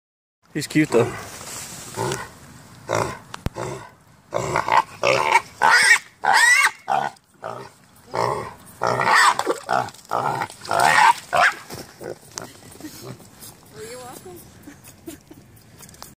Alfred Luis saves a wild hog from drowning
This little hog got stuck in a water trough and was too tired to escape.